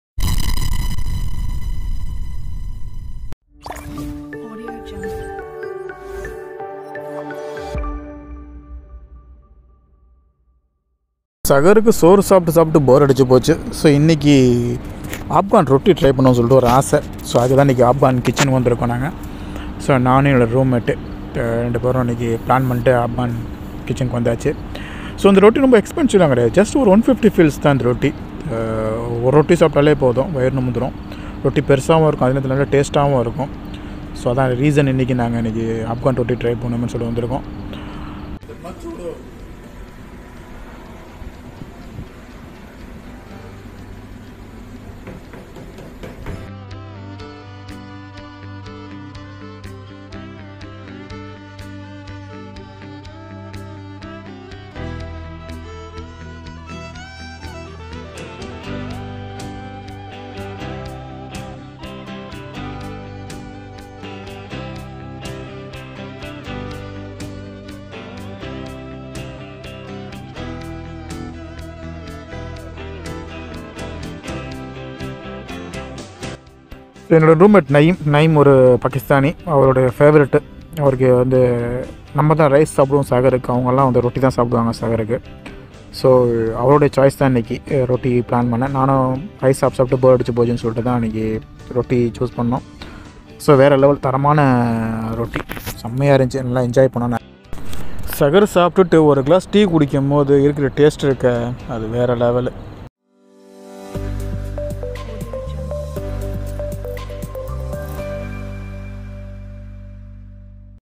Famous Afghani Roti in Dubai 🤤 | suhoor Time
Afghani Roti in Dubai
This is the Biggest chapathi/Roti which we can get in Burdubai.This outlet serves 6 different types of roti and this place is commonly used by the labours for their daily food.19₹ rupees roti and 19₹ rupees tea makes their breakfast or lunch complete.cheapest and most affordable place for food.Must try in Dubai.Afghani Roti.